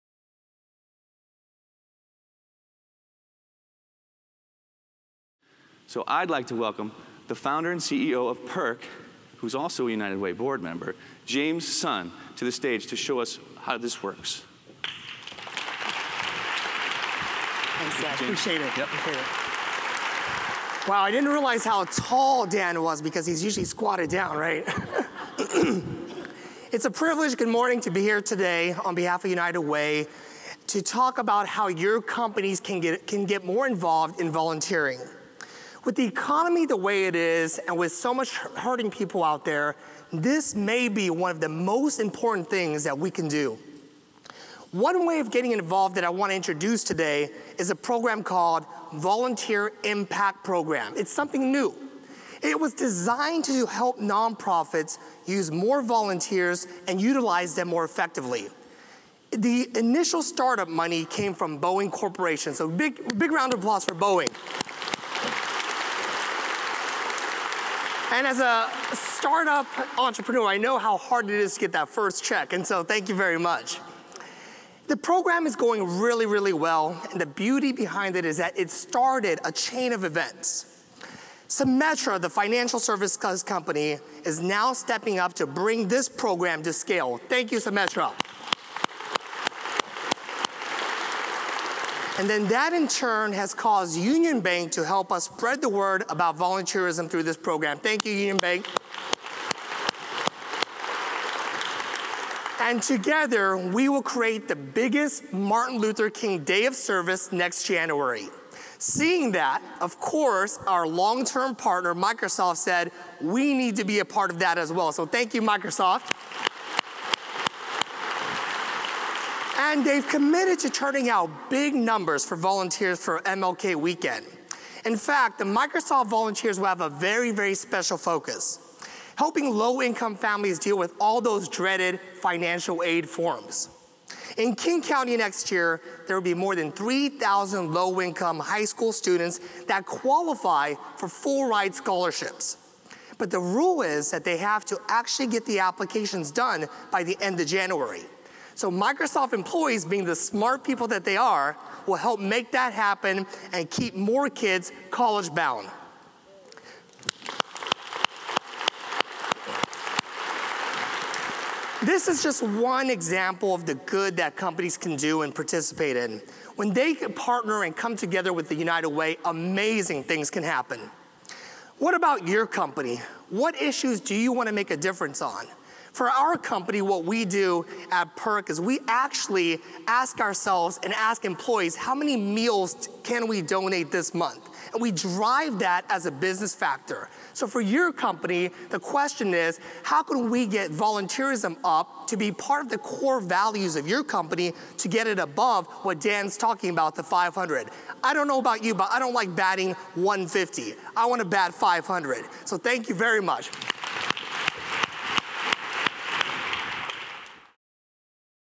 James Sun helps you get involved
James Sun, United Way of King County board member, founder of PIRQ and famous guest contestant on The Apprentice speaks on how corporations can impact our community when partnered with United Way.